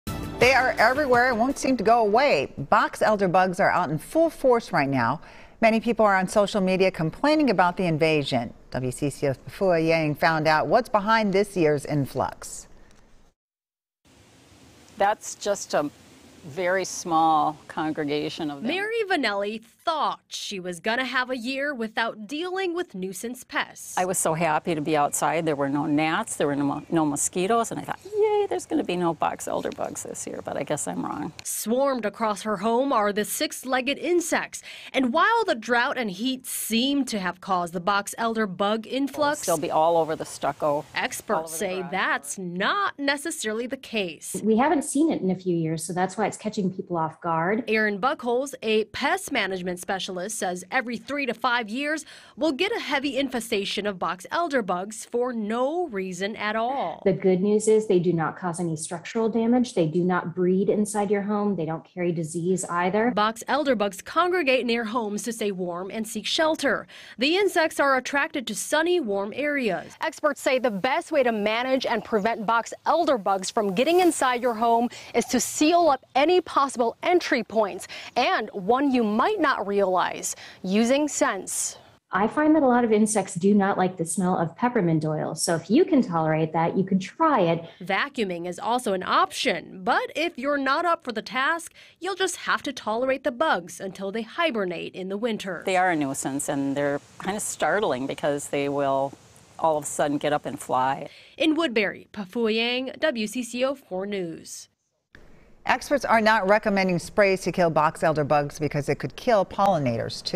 What's Behind The Influx Of Boxelder Bugs?
While the record drought and heat may seem to have caused the pest proliferation, experts say that's not necessarily the case.